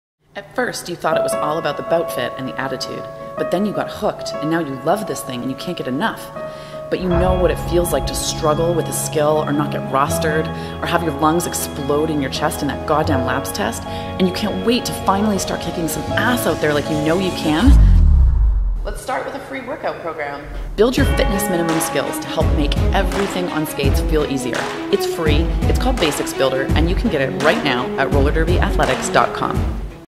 That Roller Derby Feeling...
At first you thought it was all about the boutfit...
Roller Derby Athletics' 2017 piece about why we do this thing.

Thank you to all the skaters and photographers who submitted images for this compilation!!